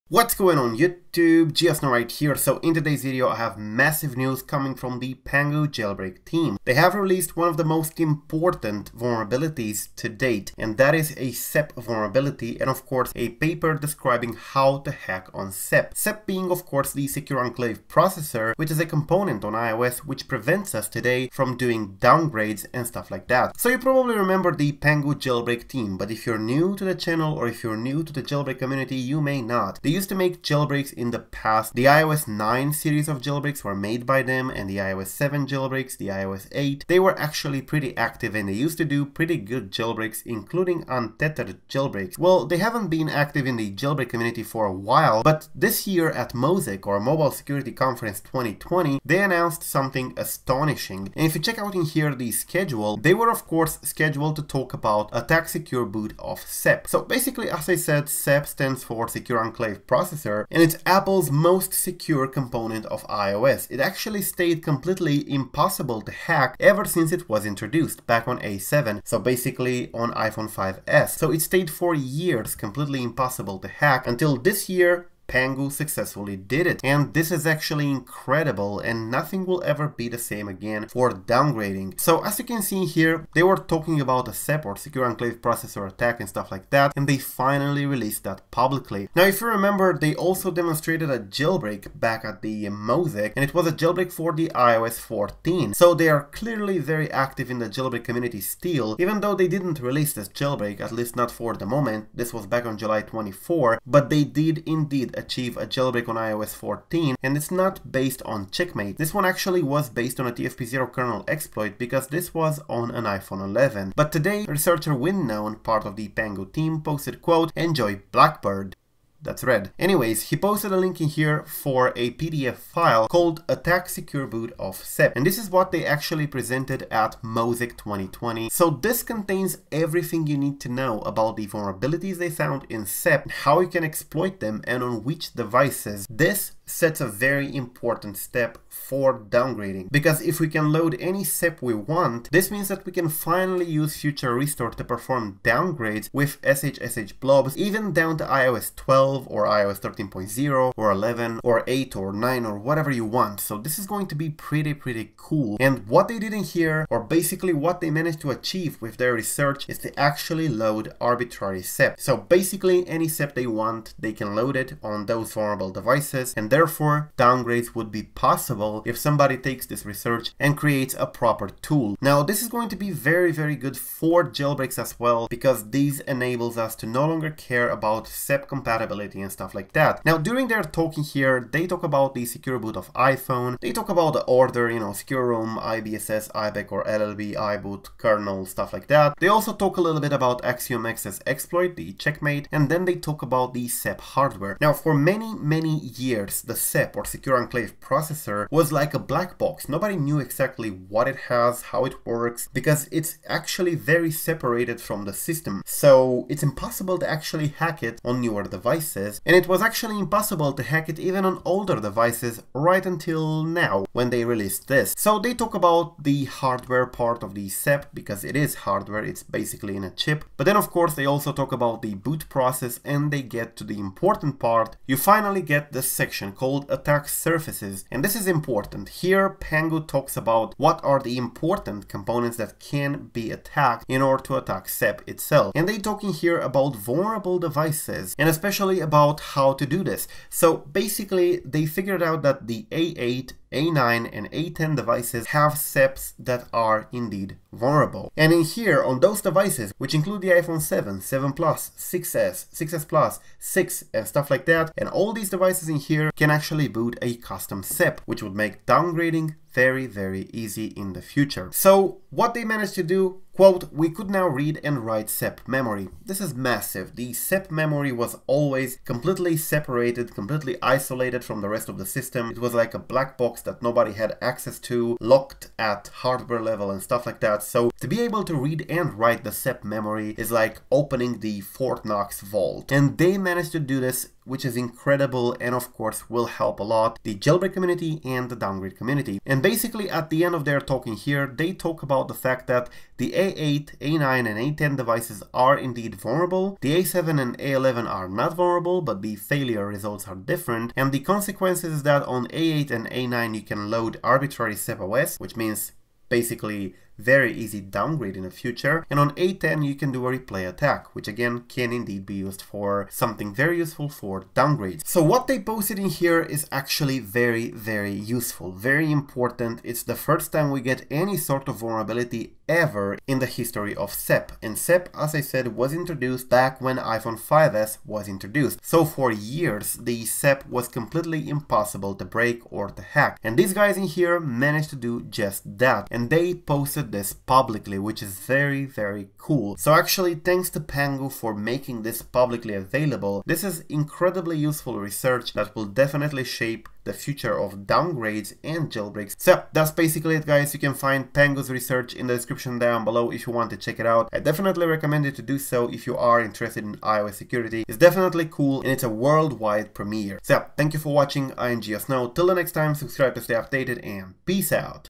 PANGU JAILBREAK TEAM Released World's FIRST iOS SEP Vuln! (GREAT For Downgrades / Jailbreak)
In today's video, we're discussing Pangu Jailbreak Team's huge release. World's first SEP vulnerabilities that can be used to load arbitrary SEPOS on certain devices. This would remove the limitations we had in terms of jailbreaks and downgrades caused by SEP incompatibility, or SEP breaking Touch ID / Face ID. This is a huge release that unveiled a lot of useful info about SEP - a component that was extremely secure on all iOS devices until Pangu came around.

SEP or Secure Enclave Processor handles Touch ID, Face ID, Passcode, Encryption, Decryption, Cryptography, etc. on your iOS device. It was considered Apple's most impenetrable component on iOS, which couldn't be hacked even when we had a full BootROM exploit released. To have publicly released exploits for it means a LOT.

~ GeoSn0w